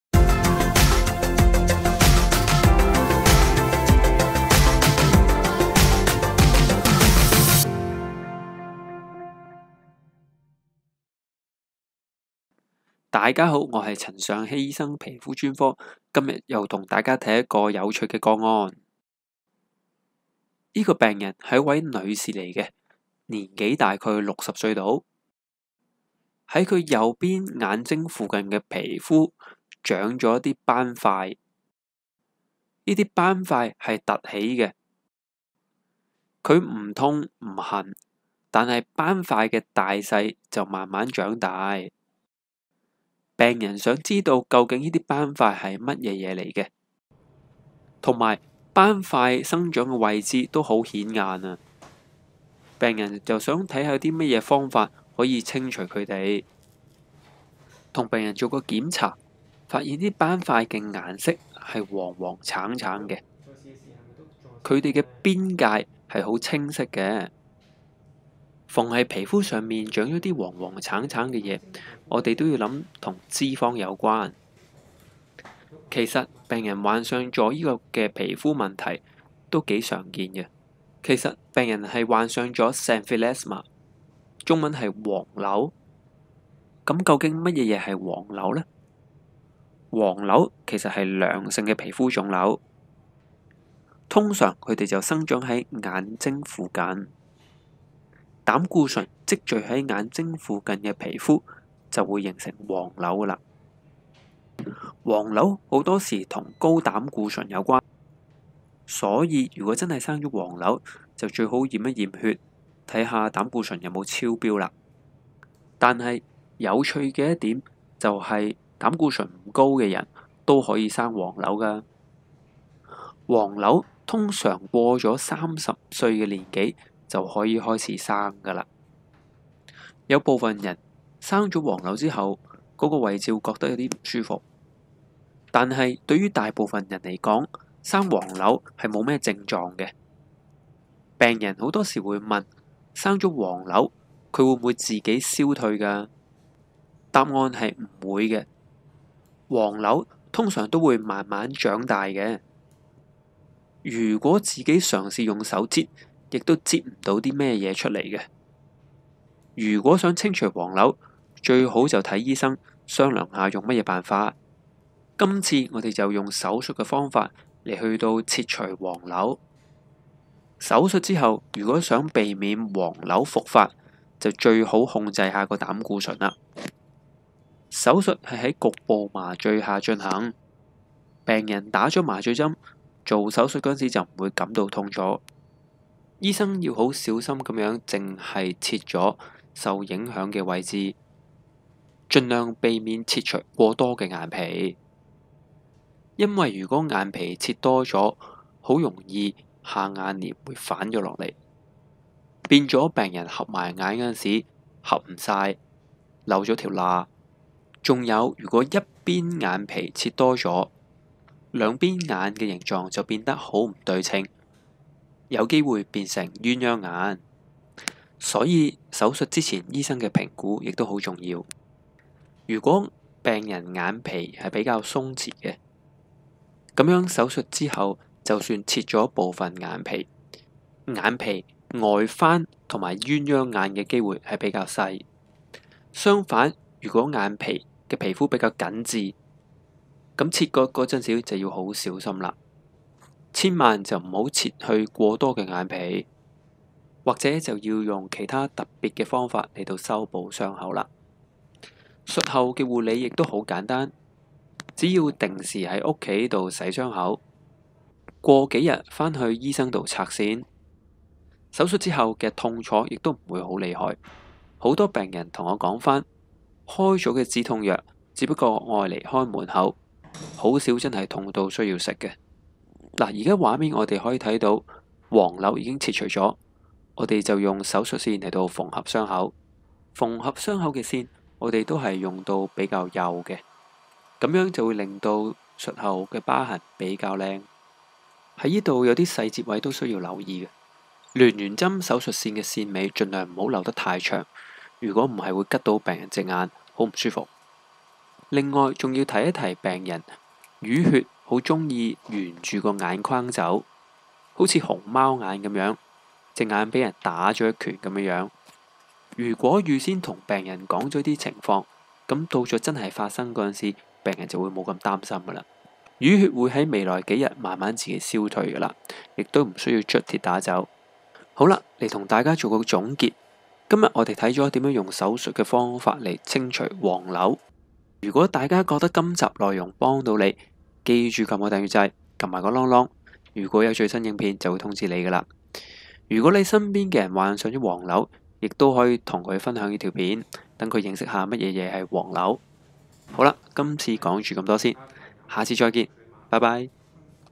黃瘤 | Xanthelasma | 完整粉瘤取出 | 表皮樣囊腫 | 皮膚活檢手術 | 陳上熙醫生 | 皮膚專科 | Dermatologist
黃瘤好多人都見過,但是未必知道它真正名稱。
究竟有黃瘤是不是一定會有高膽固醇呢? 究竟點樣處理呢?
今次就等我們一同看看怎樣用手術的方法清除黃瘤吧!

補充說明: 關於影片所示範的手術,皆存在風險。例如:流血、傷口感染、麻醉藥敏感、神經線受損、疤痕…等等。手術亦未必適用於每一個 病人。如有疑問,請和你的醫生進行詳細討論。
 這樣就不會錯過我的影片喇！